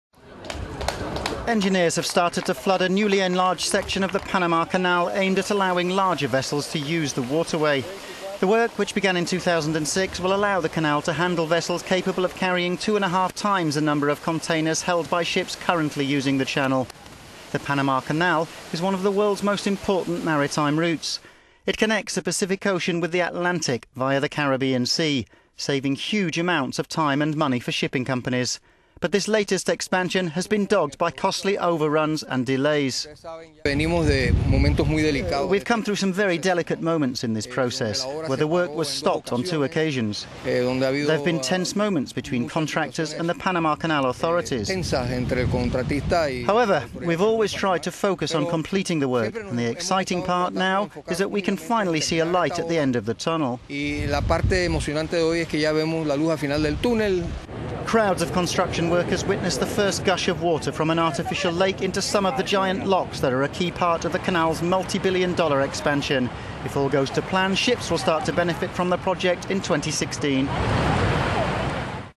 Engineers flood newly-expanded Panama Canal section
Engineers have started to flood a newly enlarged section of the "Panama Canal" aimed at allowing larger ships to use the waterway.

The work, which began in 2006, will allow the canal to handle vessels capable of carrying 2.5 times the number of containers held by ships currently using the channel.

The Panama Canal is one of the world's most important maritime routes. It connects the Pacific Ocean with the Atlantic via the Ca…